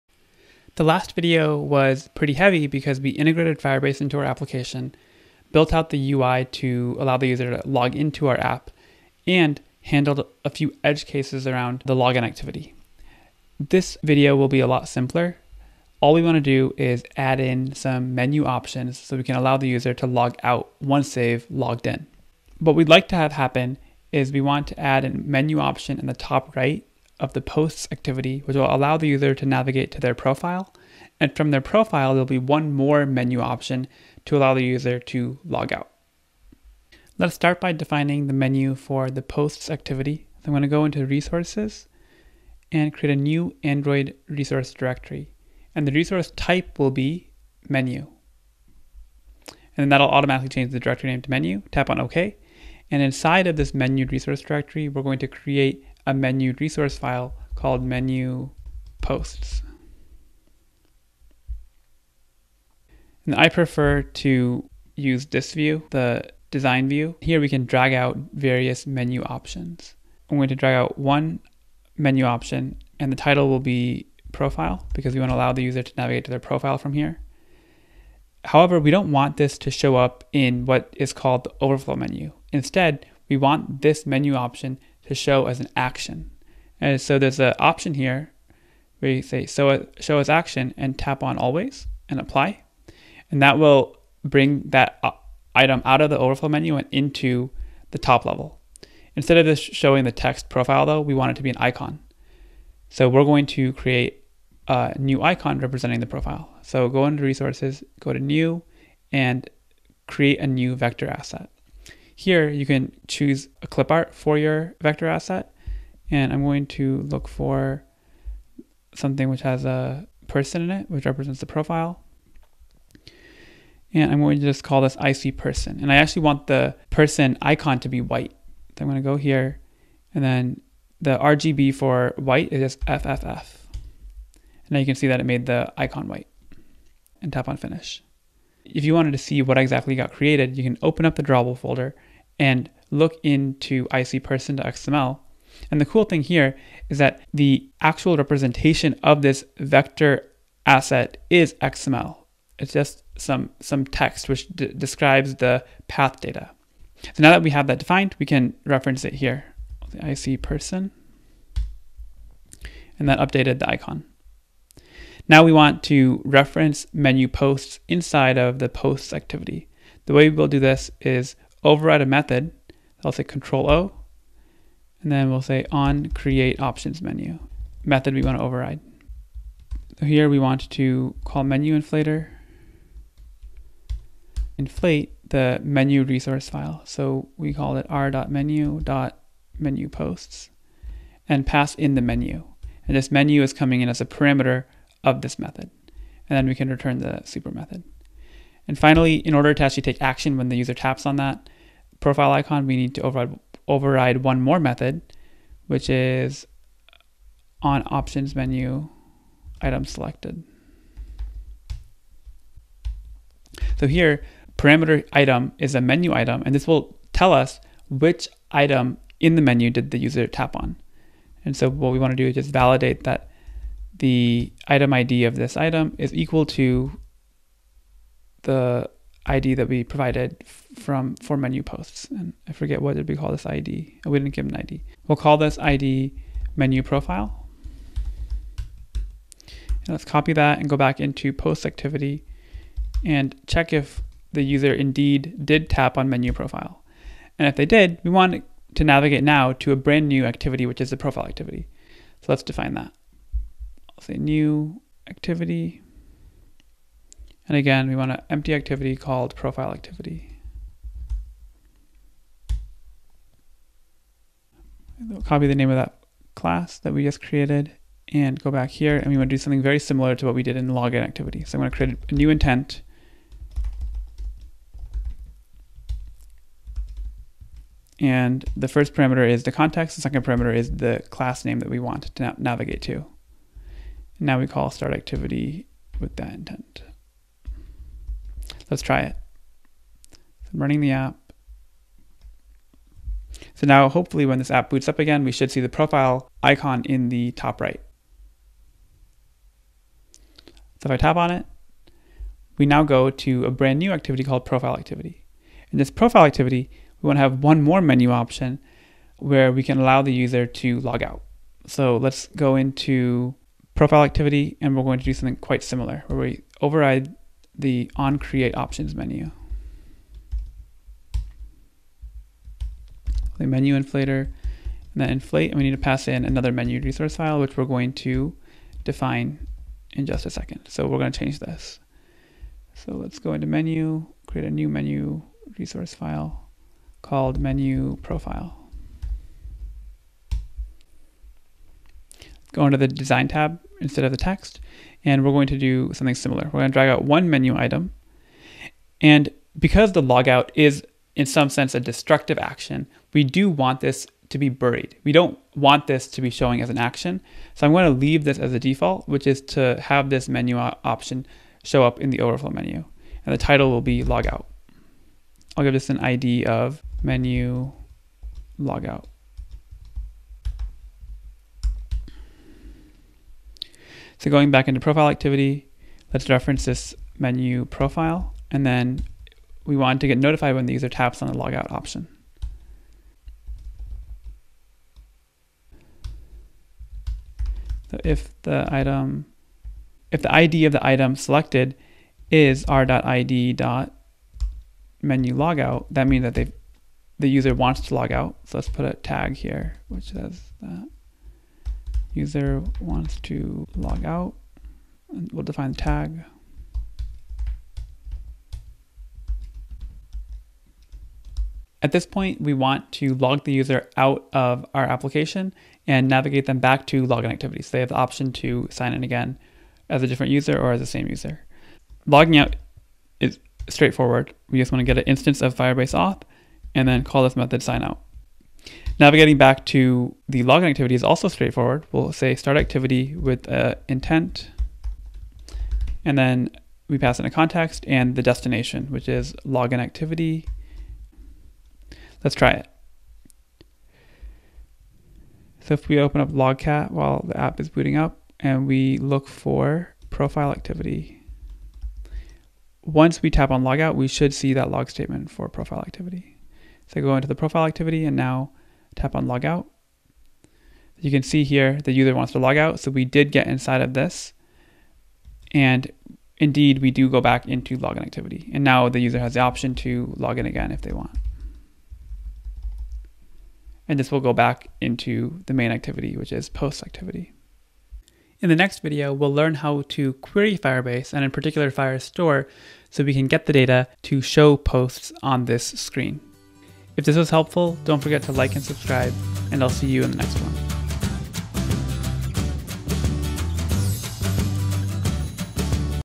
InstaFire Ep 4: Logout Option on Profile- Simple Android Instagram Clone with Firebase in Kotlin
This video (episode 4 of 12) allows the user to logout from the application. We create a profile activity, and the user has a menu option on this screen to sign out. Logging the user out is as simple as calling one method on our Firebase Authentication instance.

In these videos, we build a basic Android Instagram app clone using Firebase, written in Kotlin. Our app allows the user to sign in and create posts with an image and description. We use Firebase, an app development platform developed by Google, to power our application. We use various Firebase services such as Authentication, Storage, and Firestore to build our app.